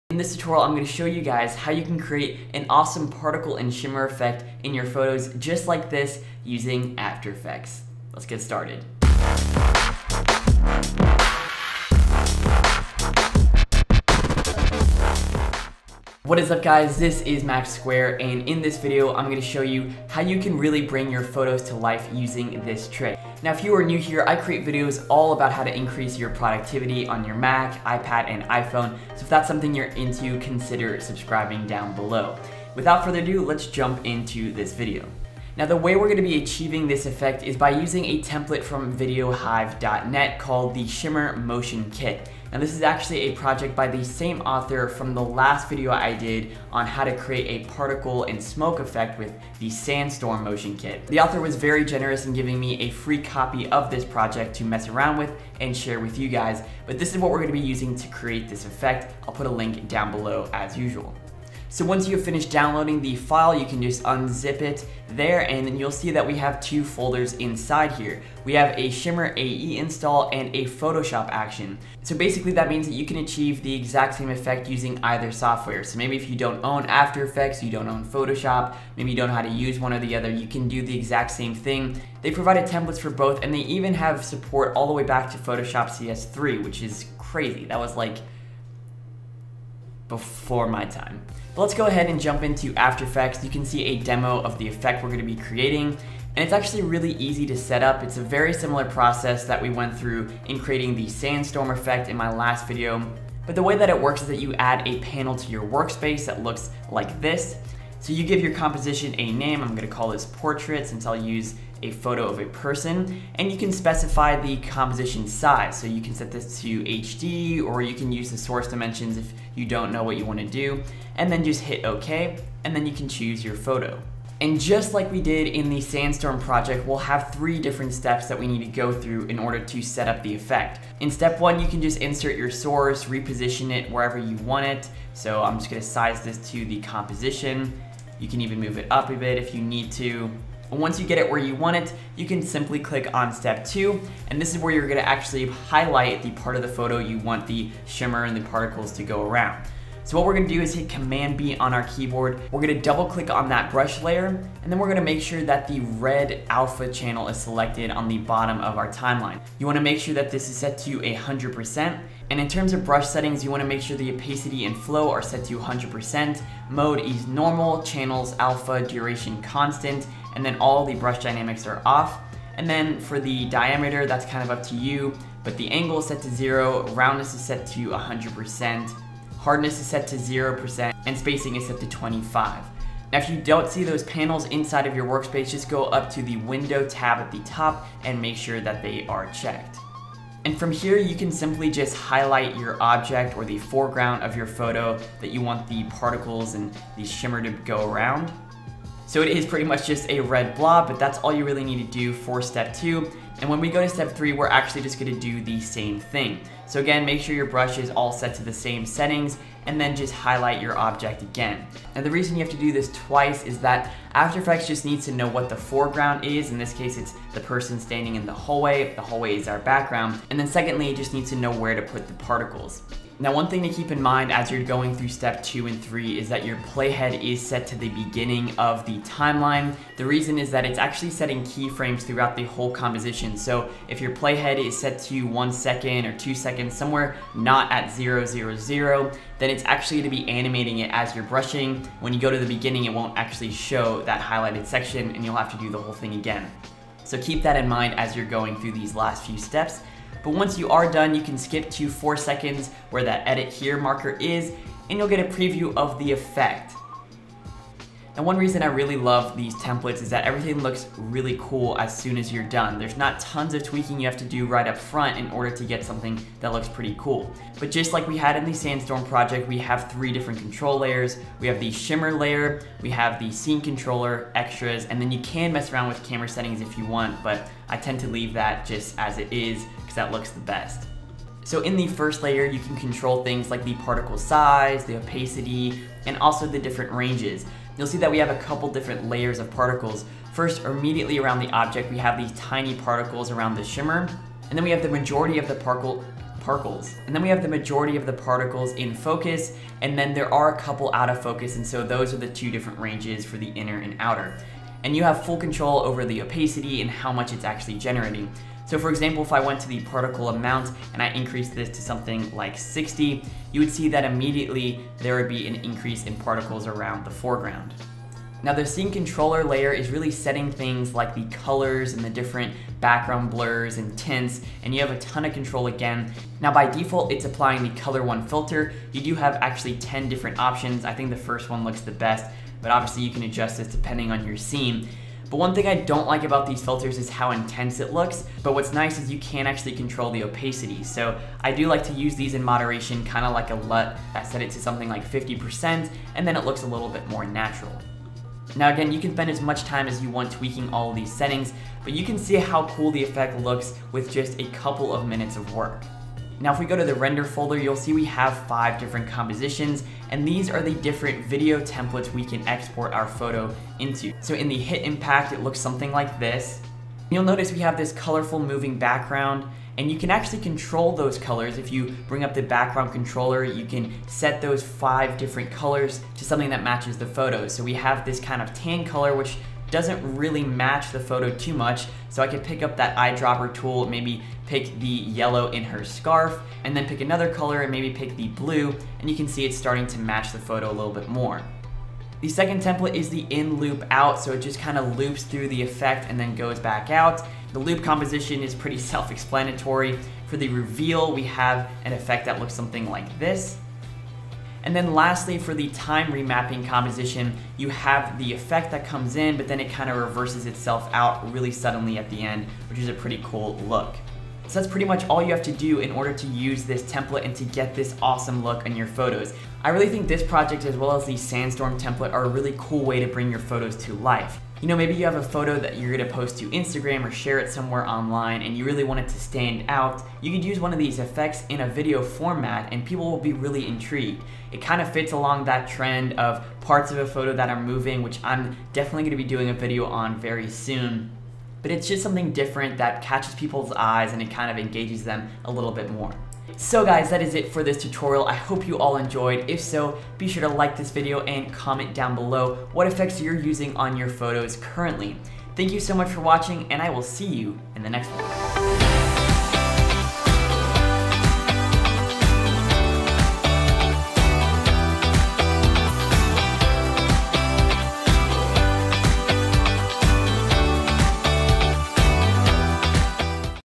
Add SHIMMER to PHOTOS! | After Effects Tutorial
In this tutorial, I'll show you how to add a shimmer and particle effect to your photos and bring them to life with this motion template. The effects takes just a couple of minutes to setup and can be used in either Adobe Photoshop or After Effects CC.

This project also includes a Photoshop action incase you do not own/want to use AE. You can still animate in Photoshop as shown in the file's tutorials which are included on the product page.

Access to BTS content & more!